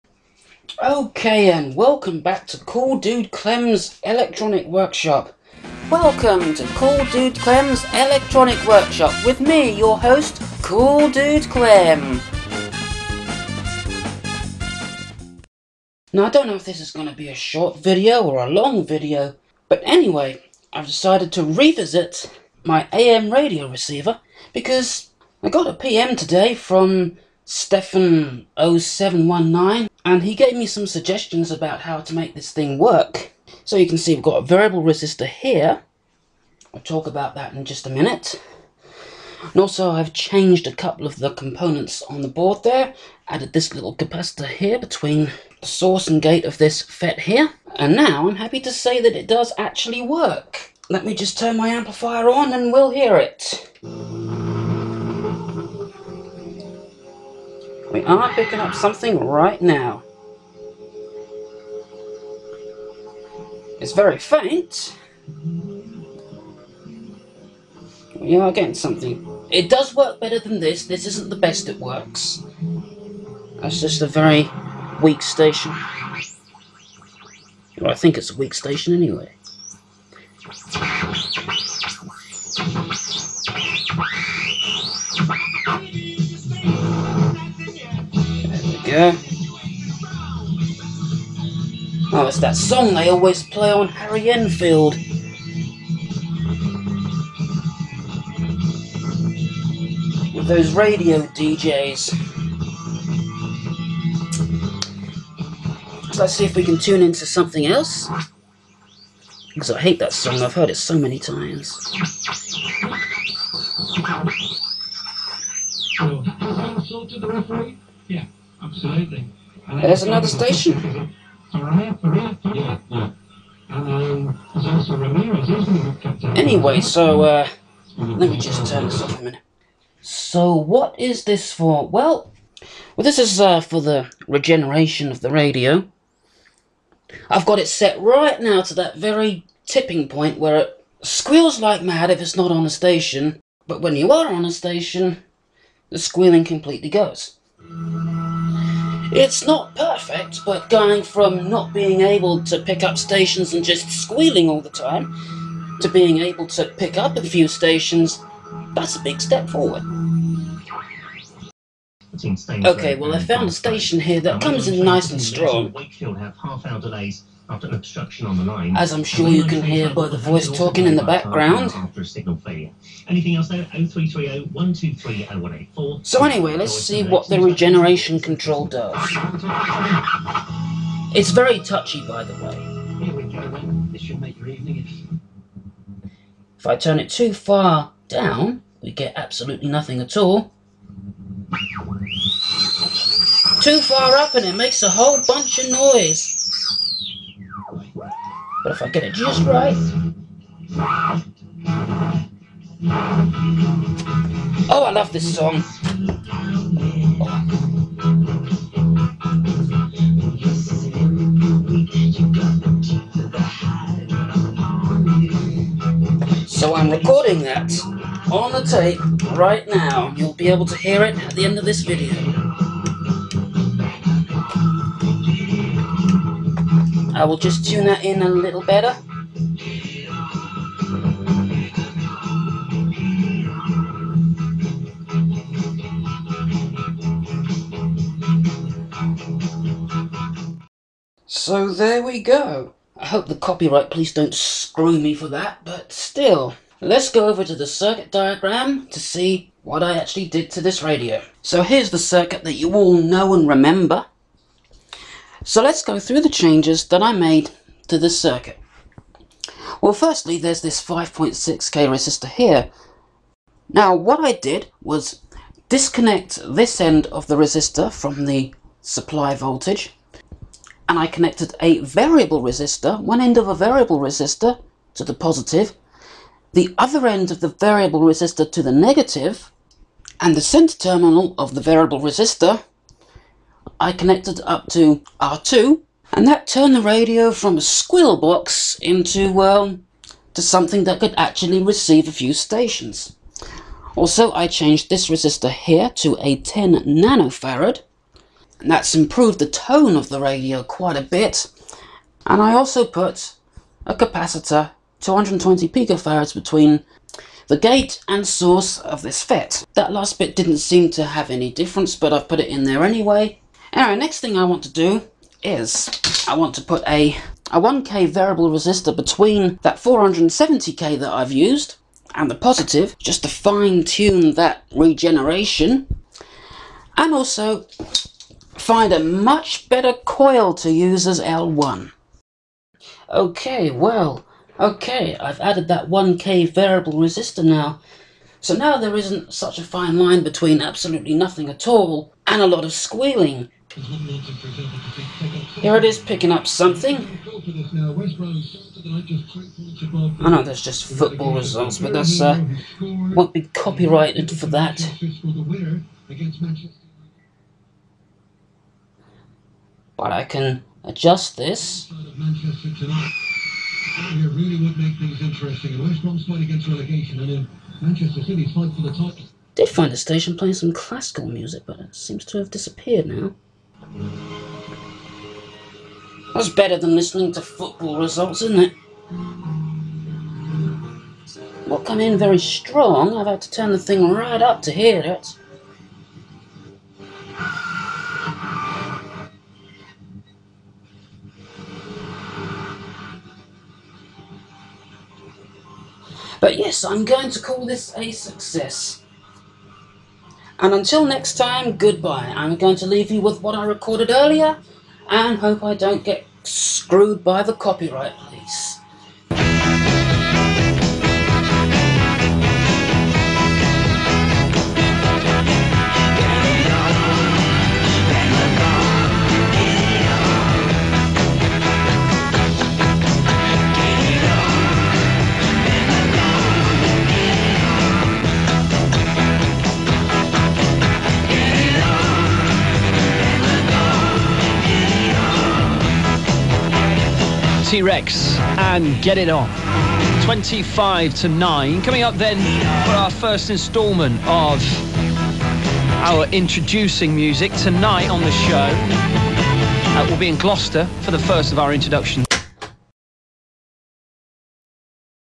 Homebrew AM Radio Success!
Thanks to some help from Stefan0719, I have it working now, it's not perfect yet, but getting it from squealing to picking up several stations is a huge step forward! All that's left is to find the ideal coil for the tuning section and then I'll move on and see if I can make a Tube version!

WMG or whoever, do NOT screw me for demonstrating the radio's sound quality, I do not claim ownership of any of the music, it is purely for demonstration purpose only.